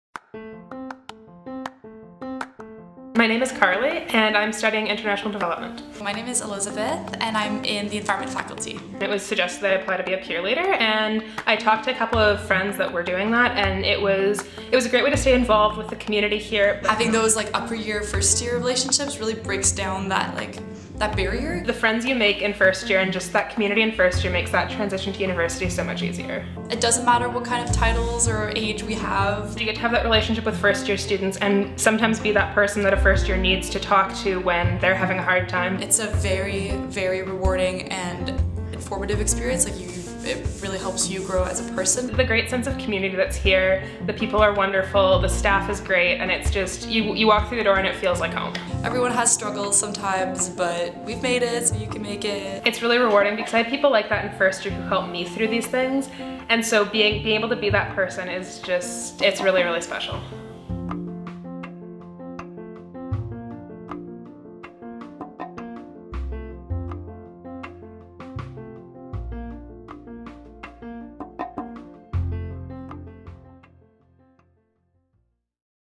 What's so great about Peer Leaders?
Are you feeling stressed out or a little overwhelmed by the craziness of first year? Looking for someone to vent to or help with strategies and advice on managing some of your work load?
Peer Leaders are upper year students who provide academic and emotional support systems for first year students. They know the ropes and have experienced the same things you have so they can understand and empathize with how things are going.
They're always there to help with resources and expertise that will help make your first year a little easier.